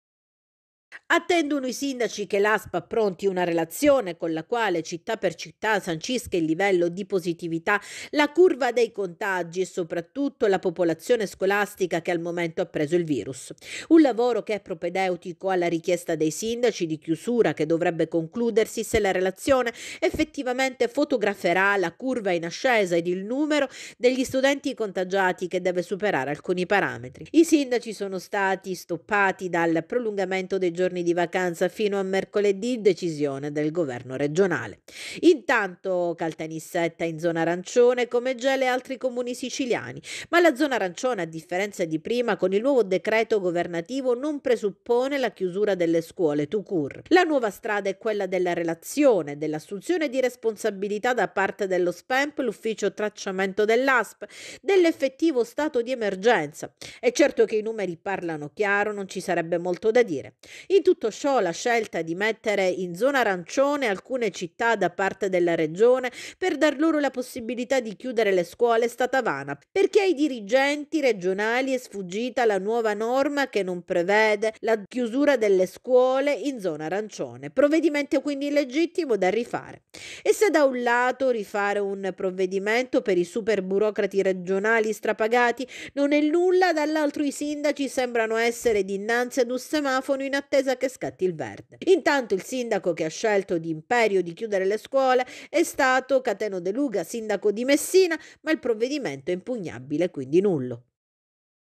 Scuole chiuse fino a mercoledì
I sindaci in attesa di indicazioni dall’Asp. Aspettano le relazioni con le analisi dell’andamento epidemiologico